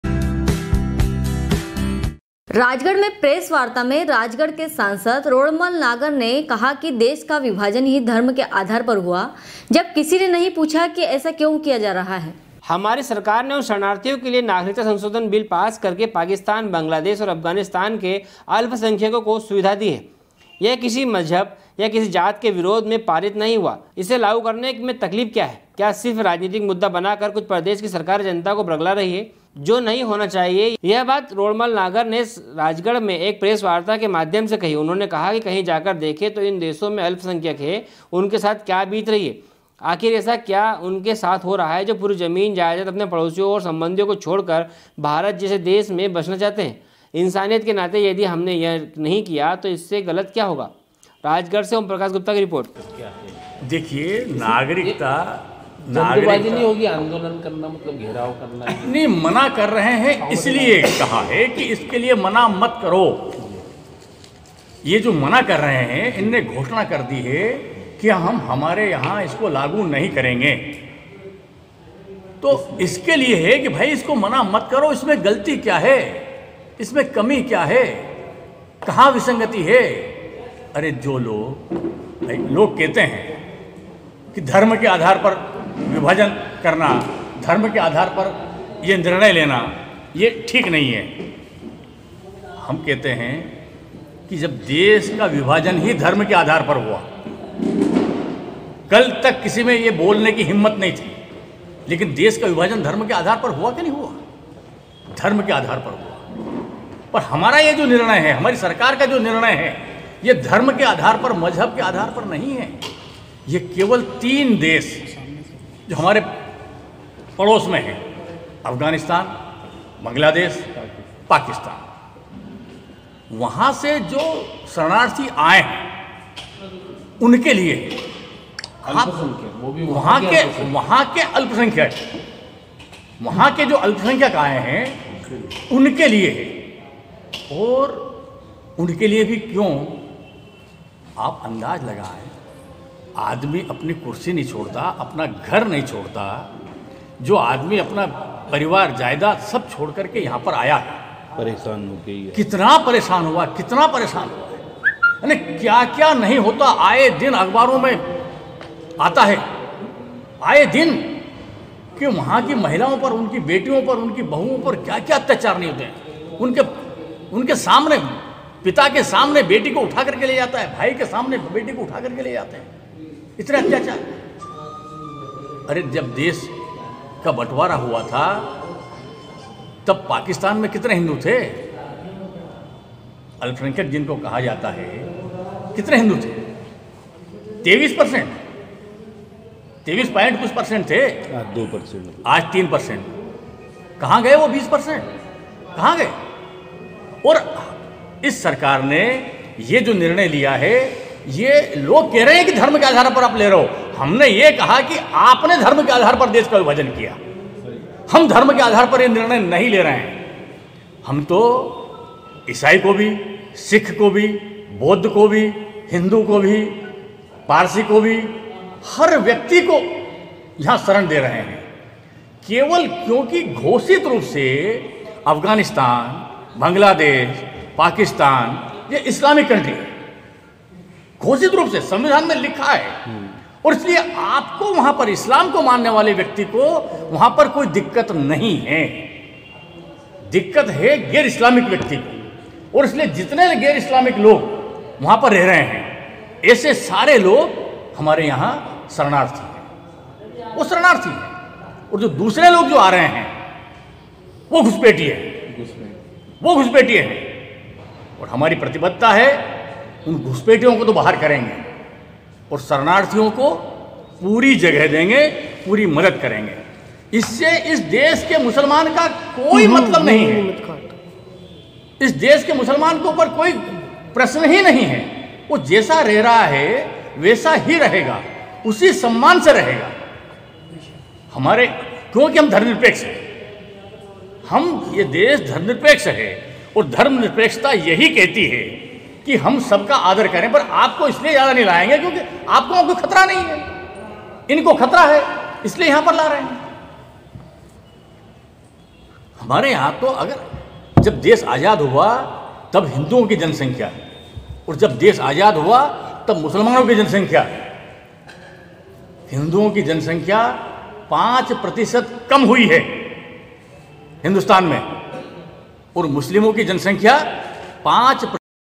sw24news rajgarh, 17.12.2019
राजगढ़ में प्रेसवार्ता में राजगढ़ के सांसद रोड़मल नागर ने कहा देश का विभाजन ही धर्म के आधार पर हुआ जब किसी ने नहीं पूछा कि ऐसा क्यों किया जा रहा है राजगढ़ से आमप्रकाष गुप्ता की रिपोर्ट#sw24news